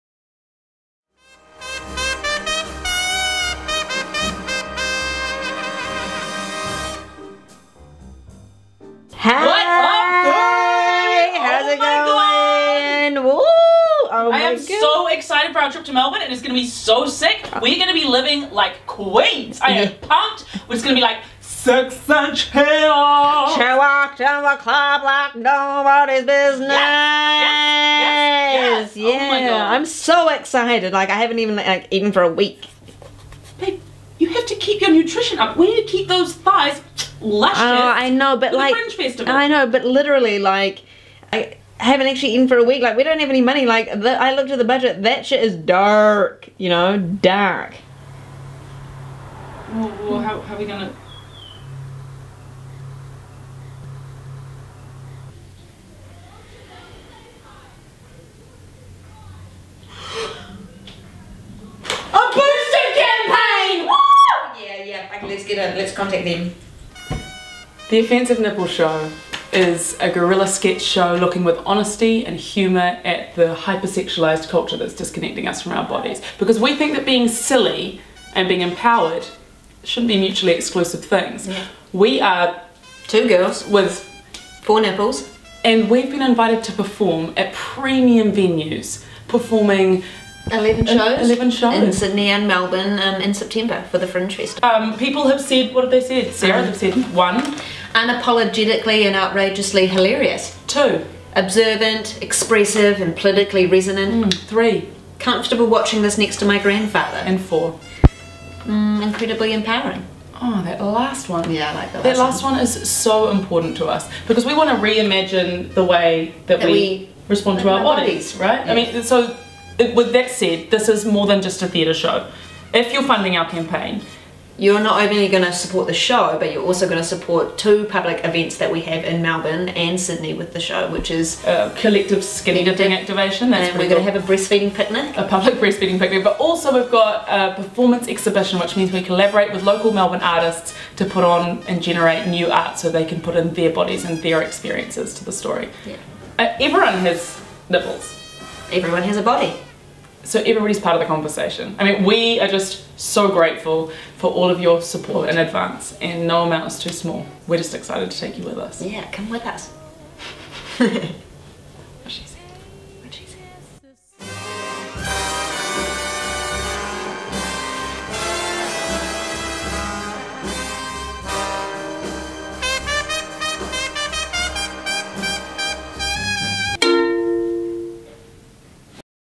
BOOST US - The Offensive Nipple Show
From the armpit of politics, comes a nipple sitcom to rival every other show you've ever seen from a breasts perspective. Winner of the Melbourne Tiki Tour Ready Fringe Award and Runner Up Best in NZ Fringe (2016), two fluorescent girls from New Zealand prepare to cross the ditch to charge through censored territory with wit and charm. The “undeniably fun” Sarah Tuck and “engaging, relentless and humorous” Jess Holly Bates (Real Fake White Dirt) perform the perfect duet to challenge the hyper-sexualised status quo. They are radical. They are silly. They are real.